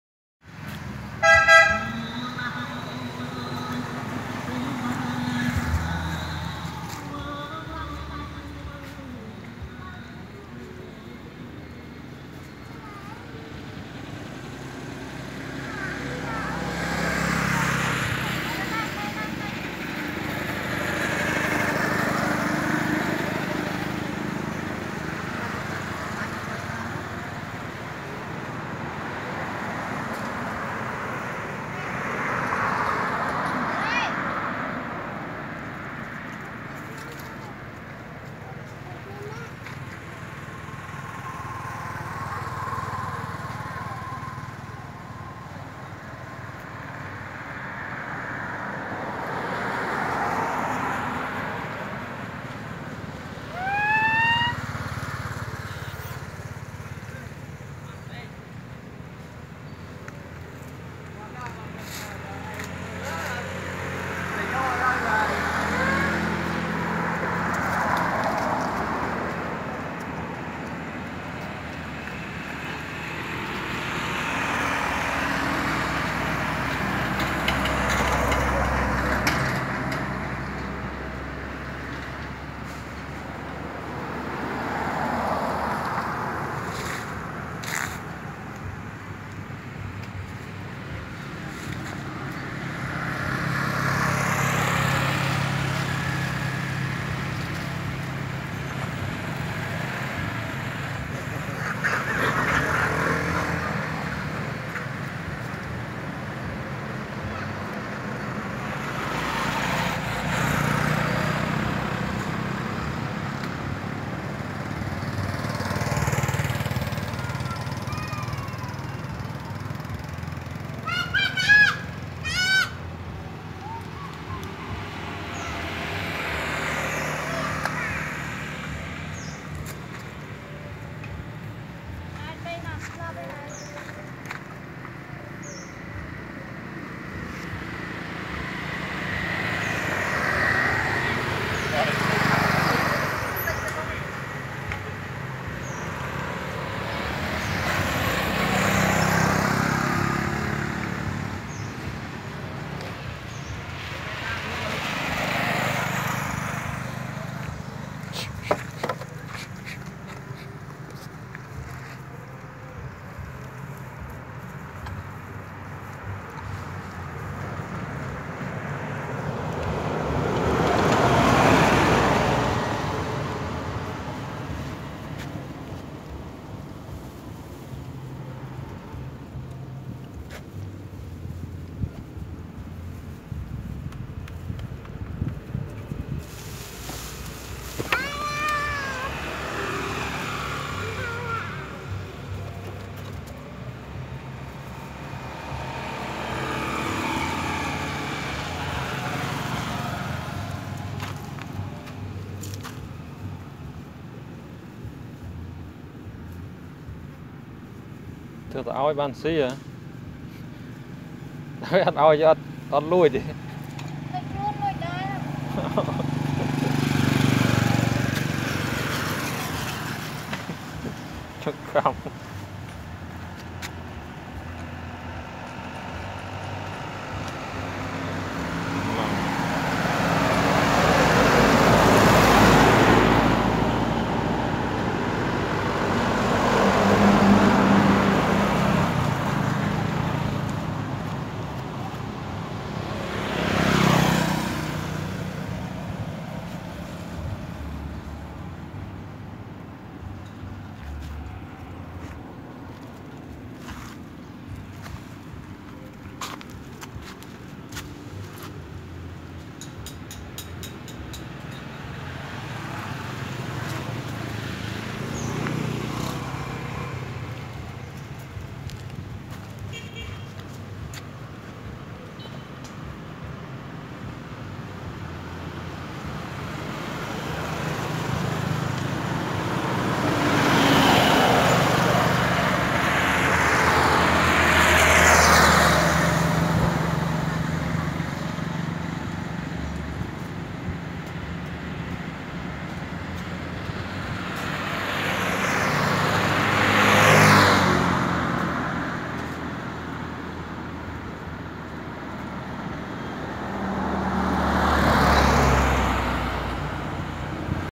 The Boy playing with The Monkey ,Some Monkey seating close to the moat of Angkor wat
The Boy playing with  The Monkey ,Some Monkey seating close to the moat of Angkor wat, Some seating on motor bike